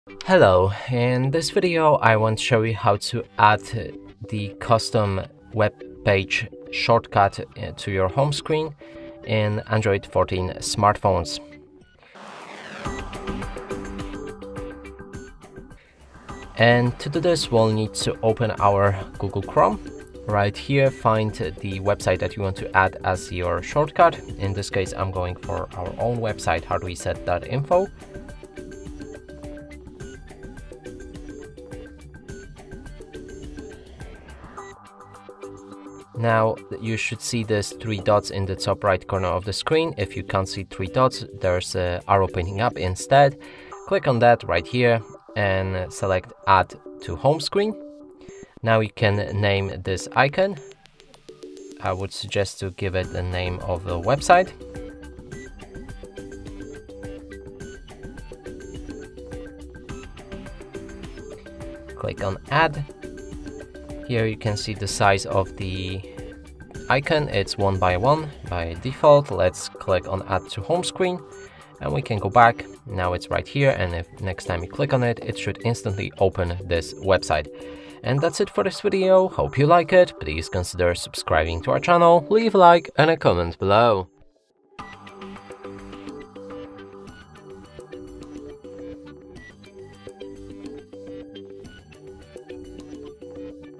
How To Add Website Shortcut to Home Screen on Android 14?
In this tutorial video, you will learn how to add a website shortcut to the home screen on Android 14. Adding a website shortcut allows you to quickly access your favorite websites with a single tap, directly from your device's home screen.

How to create a website shortcut on Android 14?
How to add a bookmark shortcut to home screen on Android 14?